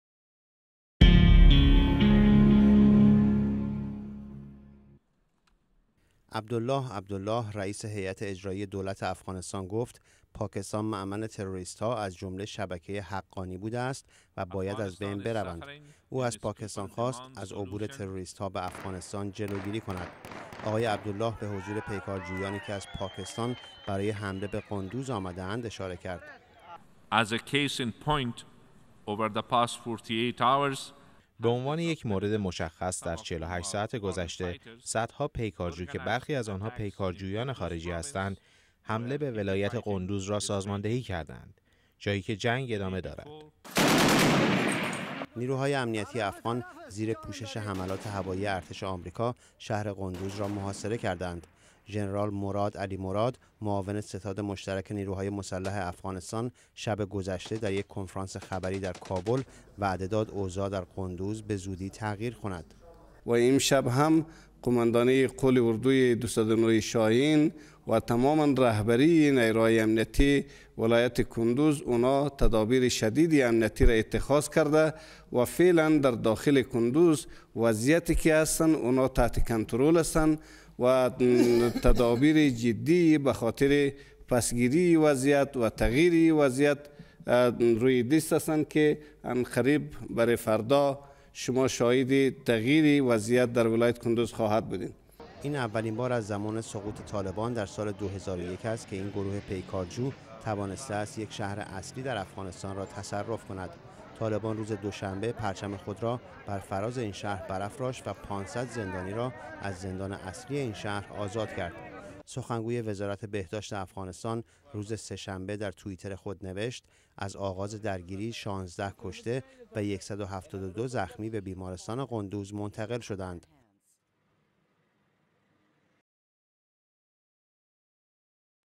عبدالله از پاکستان خواست جلوی عبور پیکارجویان به افغانستان را بگیرد
در حالی که جنگ با طالبان برای بازپس گیری شهر قندوز در افغانستان ادامه دارد، رئیس هیات اجرایی دولت آن کشور در سخنان خود در مجمع عمومی سازمان ملل متحد از پاکستان خواست از عبور پیکارجویان به افغانستان جلوگیری کند.